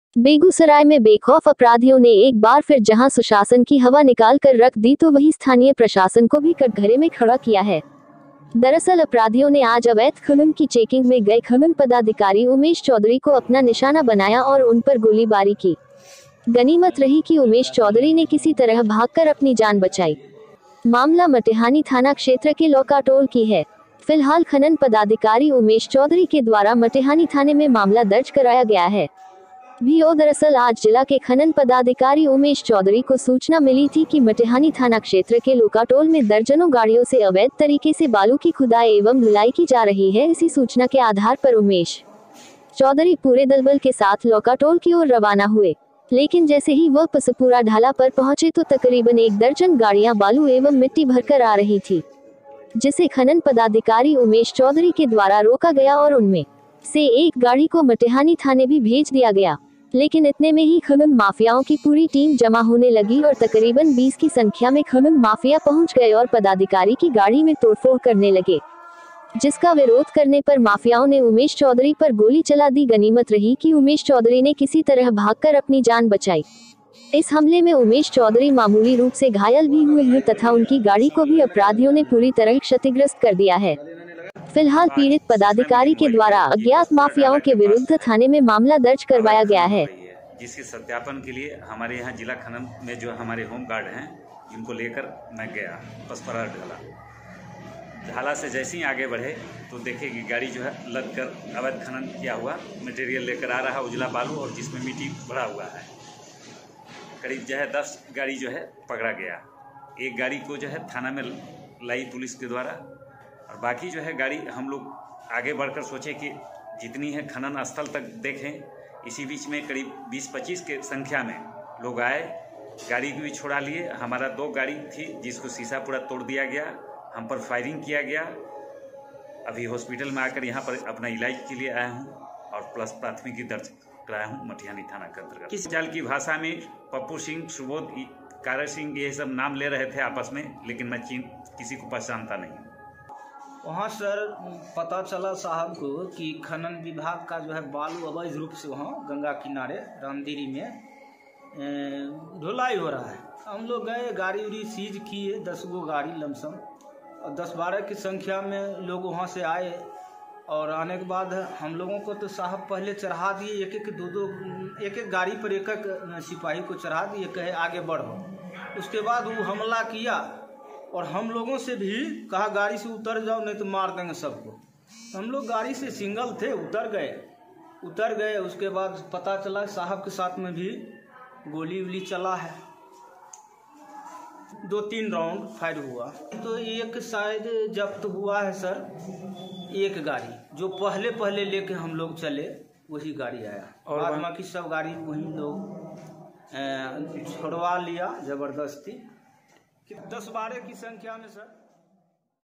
बेगूसराय में खनन पदाधिकारी पर कातिलाना हमला... वाह रे सुशासन... खतरे में अधिकारियों की जान
बेगूसराय में बेखौफ अपराधियों ने एक बार फिर जहां सुशासन की हवा निकाल कर रख दी तो वहीं स्थानीय प्रशासन को भी कटघरे में खड़ा किया है। दरअसल अपराधियों ने आज अवैध खनन की चेकिंग में गए खनन पदाधिकारी उमेश चौधरी को अपना निशाना बनाया और उन पर गोलीबारी की। गनीमत रही कि उमेश चौधरी ने किसी तरह भागकर अपनी जान बचाई ।  मामला मटिहानी थाना क्षेत्र के लौकाटोल की है। फिलहाल खनन पदाधिकारी उमेश चौधरी के द्वारा मटिहानी थाने में मामला दर्ज कराया गया है ।
भी ओ-  दरअसल आज जिला के खनन पदाधिकारी उमेश चौधरी को सूचना मिली थी कि मटिहानी थाना क्षेत्र के लोकाटोल में दर्जनों गाड़ियों से अवैध तरीके से बालू की खुदाई एवं ढ़ुलाई की जा रही है  इसी सूचना के आधार पर उमेश चौधरी पूरे दलबल के साथ लौकाटोल की ओर रवाना हुए । लेकिन जैसे ही वह पसपूरा ढाला पर पहुंचे तो तकरीबन एक दर्जन गाड़ियां बालू एवं मिट्टी भरकर आ रही थी । जिसे खनन पदाधिकारी उमेश चौधरी के द्वारा रोका गया और उनमें से एक गाड़ी को मटिहानी थाने भी भेज दिया गया । लेकिन इतने में ही खनन माफियाओं की पूरी टीम जमा होने लगी और तकरीबन 20 की संख्या में खनन माफिया पहुंच गए और पदाधिकारी की गाड़ी में तोड़फोड़ करने लगे। जिसका विरोध करने पर माफियाओं ने उमेश चौधरी पर गोली चला दी गनीमत रही की उमेश चौधरी ने किसी तरह भागकर अपनी जान बचाई। इस हमले में उमेश चौधरी मामूली रूप से घायल भी हुए हैं तथा उनकी गाड़ी को भी अपराधियों ने पूरी तरह क्षतिग्रस्त कर दिया है । फिलहाल पीड़ित पदाधिकारी के द्वारा अज्ञात माफियाओं के विरुद्ध थाने में मामला दर्ज करवाया गया है ।
बाईट- उमेश चौधरी - खनन पदाधिकारी -बेगूसराय
बाईट -अशोक कुमार - सिपाही चश्मदीद